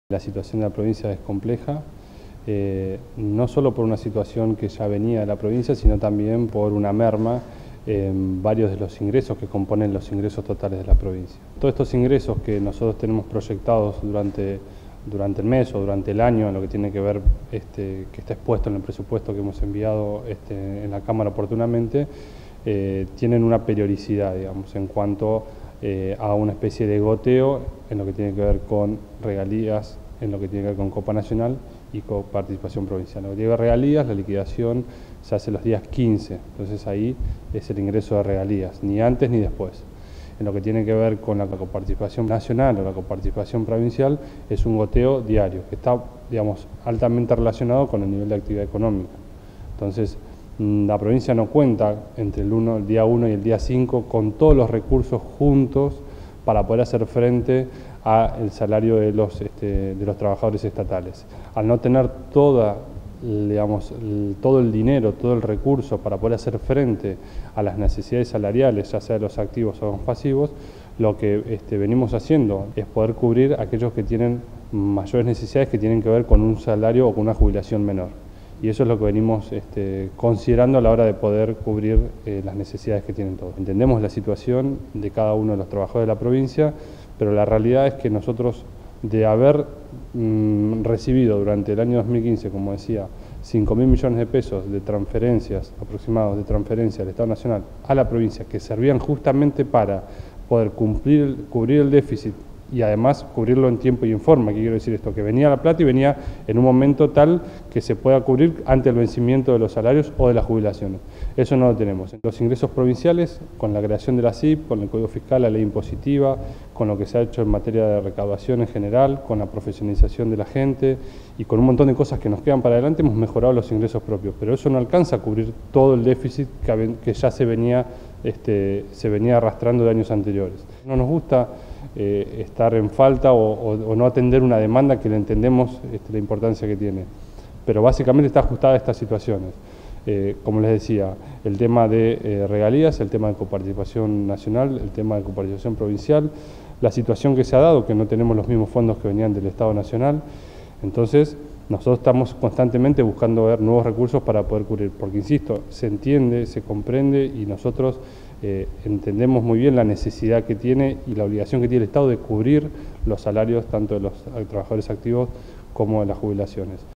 Donnini: “Estamos buscando permanentemente recursos para cubrir el déficit”
Así lo afirmó el ministro de Economía, Finanzas e Infraestructura de la provincia, Juan Donnini al finalizar una mesa de trabajo que mantuvo junto a la ministra de Desarrollo Social, Paola Vessvessian y el presidente de la Caja de Previsión Social, Ariel Ivovich. “Entendemos la situación de los trabajadores”, resaltó el ministro.

Asimismo, el titular de la cartera económica provincial dio detalles de la situación de la provincia a la que calificó como “compleja, no sólo por una situación que ya venía en la provincia sino también por una merma en los ingresos totales de la provincia” y añadió que estos ingresos mensuales y anuales  “que están expuestos en el presupuesto que hemos enviado a la Cámara oportunamente, tienen una especie de goteo en lo que tiene que ver con regalías y con la  coparticipación nacional y provincial”.
En este sentido, detalló “la liquidación las regalías se hace los días 15, ni antes ni después, y en lo que tiene que ver con coparticipación nacional o coparticipación provincial, es un goteo diario donde está altamente relacionado con el nivel de actividad económica”.
También destacó que los ingresos de la provincia son diarios. “La provincia no cuenta entre el día 1 y el día 5, con todos los recursos juntos para poder hacer frente a todos los salarios de los trabajadores estatales”.
Y agregó: “Al no tener todo el dinero junto para poder hacer frente a las necesidades salariales tanto a los activos como a los pasivos, lo que venimos haciendo es poder cubrir aquellos sectores que tienen mayores necesidades que tiene que ver con un salario o una jubilación menor, y eso es lo que venimos consideramos a la hora de poder cubrir las necesidades que tienen todos”.
En otro tramo, hizo hincapié en que el Gobierno Provincial comprende “la situación de cada uno de los trabajadores pero la realidad es que durante el año 2015, recibimos 5 mil millones de pesos de transferencias del estado nacional a la provincia que servían justamente para poder cubrir el déficit y además cubrirlo en tiempo y forma”. Es decir que “venía la plata y venía en un momento tal que se pueda cubrir antes del vencimiento  de los salarios, de las jubilaciones, y eso no lo tenemos”.
En cuanto a los ingresos propios, el ministro señaló: “Con la ASIP, la ley impositiva, con lo que se ha hecho en materia de recaudación en general, con la profesionalización de la gente, y con un montón de cosas que nos quedan para adelante, hemos mejorado los ingresos propios, pero eso no alcanza a cubrir todo el déficit que ya se venía arrastrando  en años anteriores, no nos gusta estar en falta o no atender una demanda que entendemos la importancia que tiene, pero básicamente está ajustada a esta situaciones”.
“Nosotros estamos constantemente  buscando nuevos recursos para poder cubrir, - porque insisto – se entiende , se comprende y nosotros  entendemos muy bien la necesidad que tiene y la obligación que tiene el Estado para cubrir los salarios tanto de los trabajadores activos como el de las jubilaciones”, subrayó Donnini.
Por su parte, el presidente de la Caja de Previsión Social, Ariel Ivovich resaltó que al día de la fecha se cumplió con el 60 por ciento de los beneficiarios y resaltó que desde el Gobierno de Santa Cruz “estamos trabajando todos los días para poder lo antes posible cumplir con la totalidad de los pagos”.
“Lo cierto es que los recursos genuinos, que son los aportes y contribuciones de  los trabajadores activos para el sostenimiento del pago de las jubilaciones, más las leyes especiales y otros recursos que ingresan a nuestra Caja, no alcanzan para pagar la totalidad de las jubilaciones”, indicó Ivovich.
En este sentido, informó: “Tiene un costo el sostenimiento de nuestra Caja y ese costo es el déficit que todos los meses el Ministerio de Economía nos tiene que girar para que nosotros podamos cumplir con la totalidad de nuestros compromisos” al tiempo que resaltó: “Nuestra Gobernadora ha manifestado en varias oportunidades que está dispuesta a sostenerla (la Caja de Previsión Social)”.
Por último, la ministra de Desarrollo Social, Paola Vessvessian hizo hincapié en que al Gobierno provincial: “También nos importa cada uno de los jubilados en la situación en que está y sobre todo aquellos que hoy están en la puerta de la Caja de Previsión Social”.
A la vez que, enfatizó: “Estamos permanentemente acercándonos y dialogando para poder ver algunas otras prácticas de reclamo que no pongan en riesgo las situaciones de cada uno de los jubilados”.